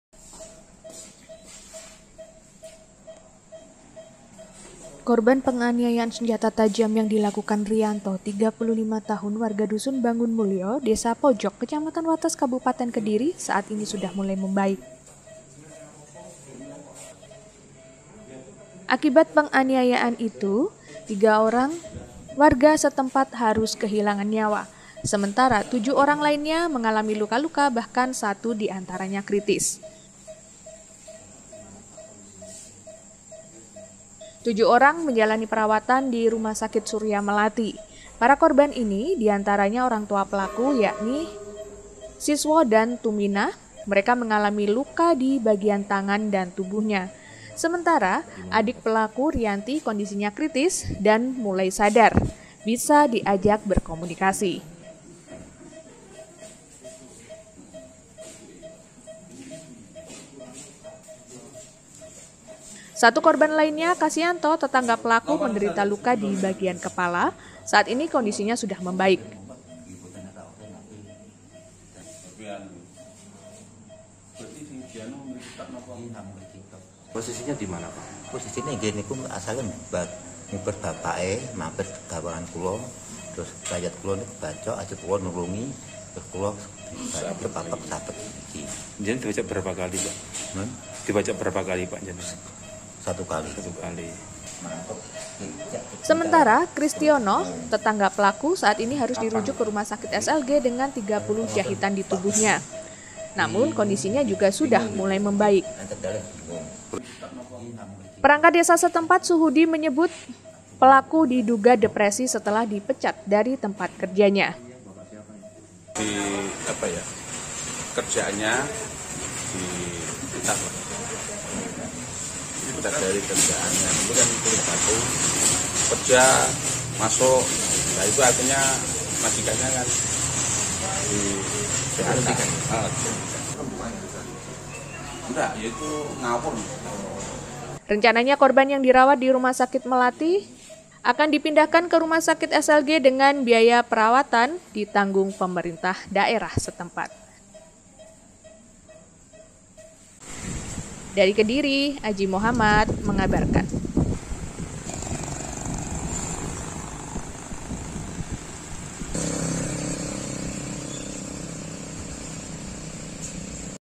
Korban Luka Akibat Penganiayaan Riyanto Kondisinya Membaik
Korban penganiayaan senjata tajam yang dilakukan oleh Riyanto (35 tahun) warga Dusun Bangunmulyo Desa Pojok Kecamatan Wates Kabupaten Kediri saat ini sudah mulai membaik.

Akibat penganiayaan itu, 3 orang warga setempat harus kehilangan nyawa. Sedangkan 7 orang mengalami luka-luka bahkan 1 diantaranya kritis.

7 Korban menjalani perawatan di Rumah Sakit Surya Melati. Para korban ini diantaranya orang tua pelaku yakni Siswo dan Tuminah. Mereka mengalami luka di bagian tangan dan tubuhnya. Sementara adik pelaku, Riyanti yang kondisinya kritis saat ini sudah sadar dan bisa diajak berkomunikasi.

Satu korban lainnya adalah Kasiyanto, tetangga pelaku yang menderita luka di bagian kepala kondisinya juga membaik.

Satu orang korban lainnya, Kristiono dirujuk ke RS SLG dengan 30 jahitan di tubuhnya. Saat ini sudah membaik.

Menurut perangkat desa setempat, Suhudi, diduga pelaku ini depresi setelah dipecat dari tempatnya bekerja.

Rencananya korban yang dirawat di RS Surya Melati akan dipindahkan ke RS SLG dengan biaya perawatan ditanggung pemerintah. *

Dari Kediri, AJi Muhammad Mengabarkan